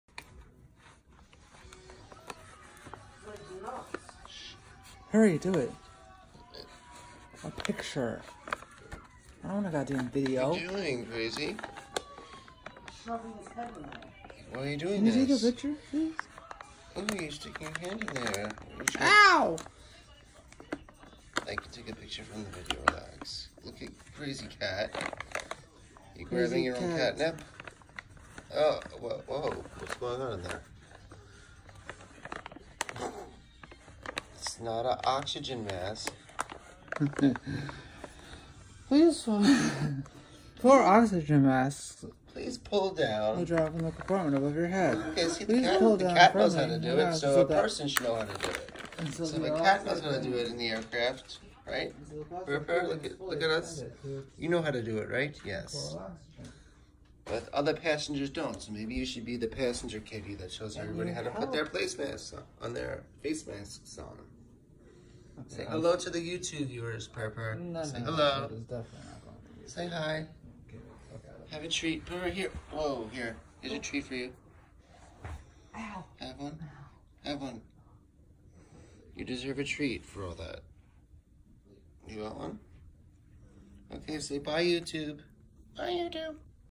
Flight attendant Kitty demonstrates correct use of oxygen mask
My crazy cat smelling his catnip in a bag but it looks like he’s using an oxygen mask or a bag to breath in and out air, lol he’s so funny and crazy. He’s a huge main coin very long hair I found him as a kitten when I lived in a Fairfield property and I decided to keep him but he does a lot of tricks. If you’re interested to see other things he can do beside breath in and out of a bag lol let me know haha, he can do tricks.